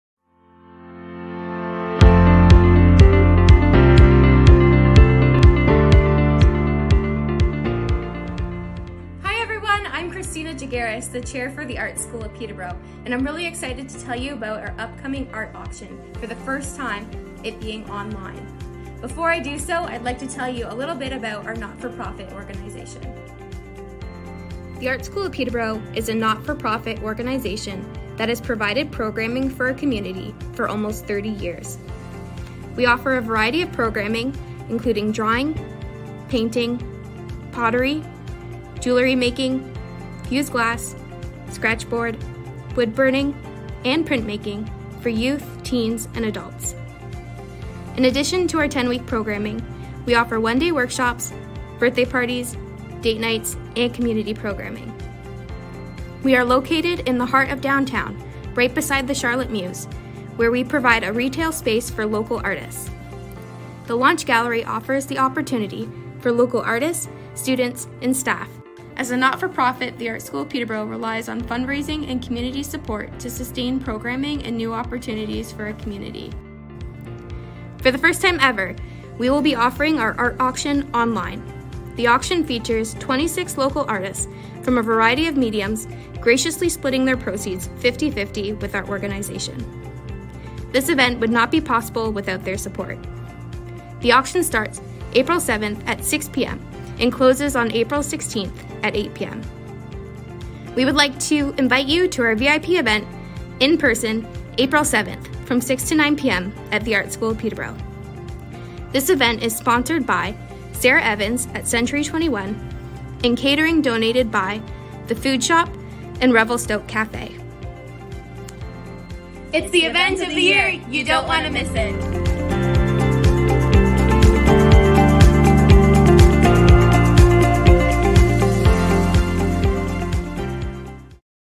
2023 Online Art Auction
The Art School of Peterborough will be hosting its first online Art Auction April 7 at 6pm until April 16 at 8pm. Artwork can be viewed here: